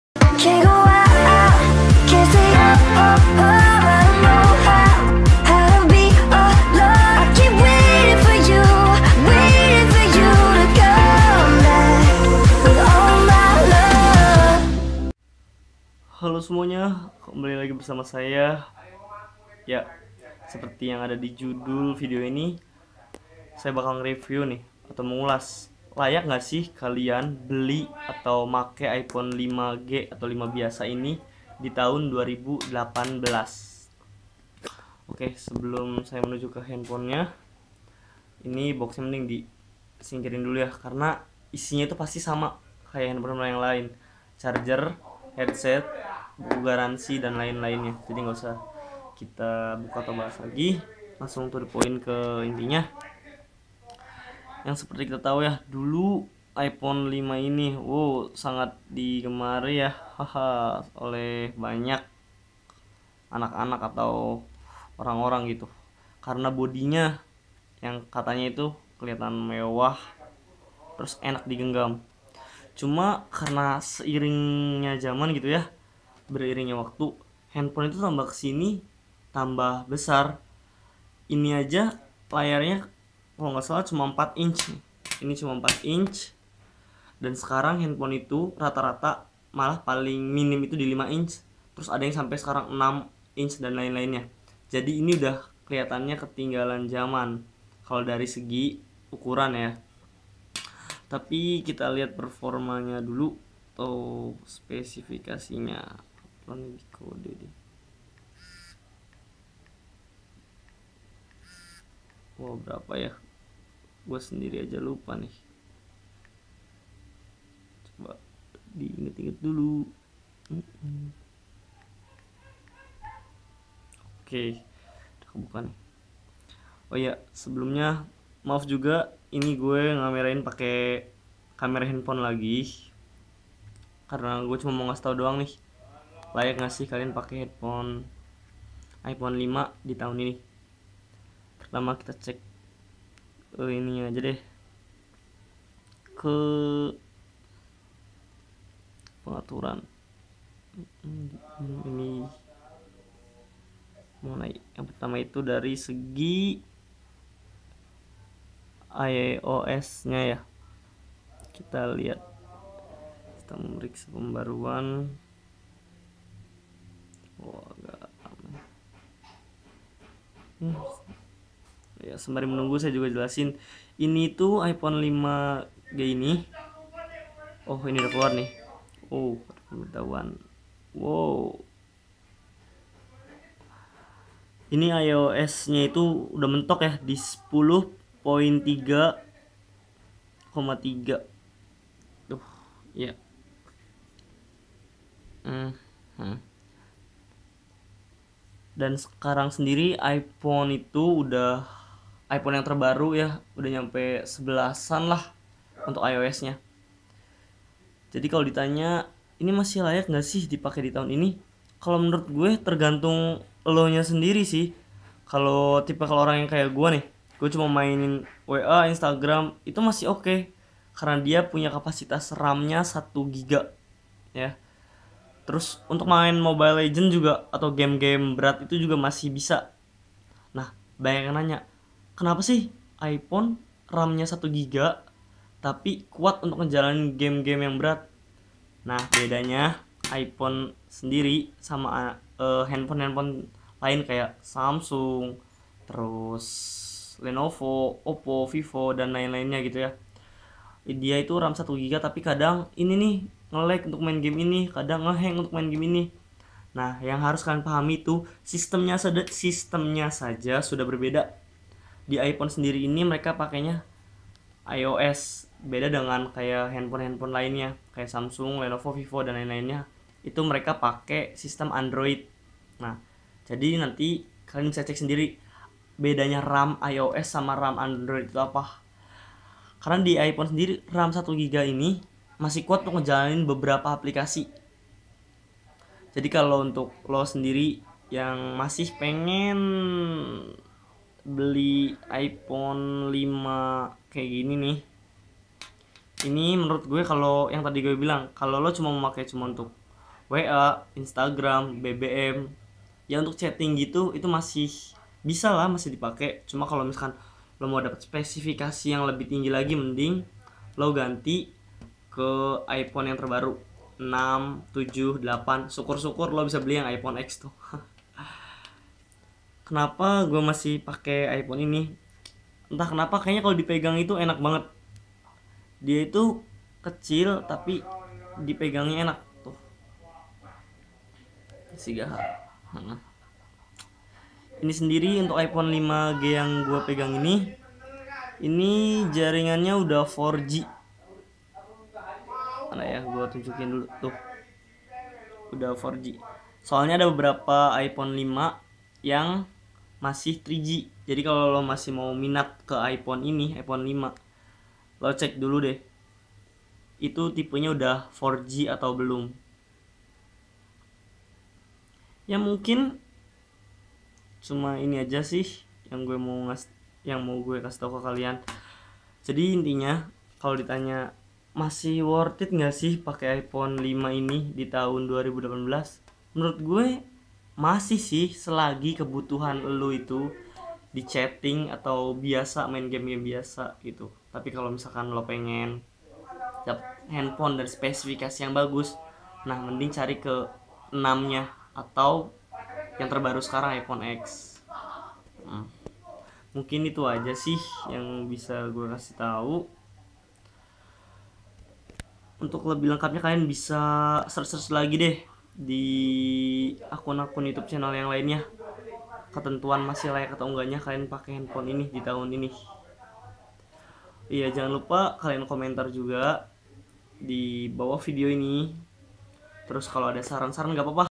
Beli iphone 5 di tahun 2020, masih layak ga sih?
Kali ini saya ingin sedikit memberitahu mengenai iphone 5g 16gb, apakah masih layak pakai di tahun 2018?iphone 5g,

.untuk spesifikasi lebih jelas, kalian bisa cek disini.
Mhon maaf jika hasil/kualitas videonya kurang HD.
Silahkan berikan kritik dan saran di bawah 👇 ini.